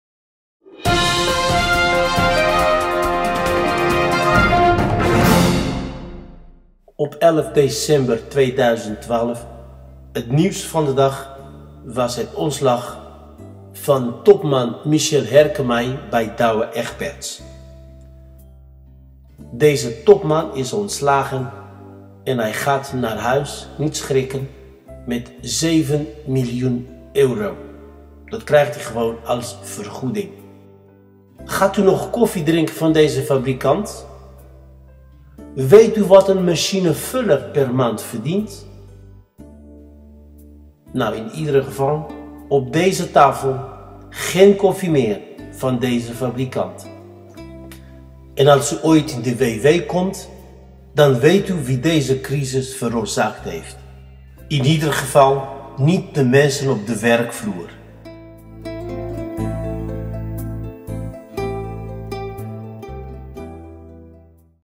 Ontslagvergoeding van 7 miljoen
Het ontslag van Michiel Herkemij bij Douwe Egberts legt hem geen windeieren. Zijn vertrek kan hem 2,25 miljoen euro opleveren. Tel daar de 4,7 miljoen euro bij op waarvoor Herkemij in zijn eerste -- en enige -- jaar bij DE in de boeken stond, en het totaal komt op bijna 7 miljoen euro.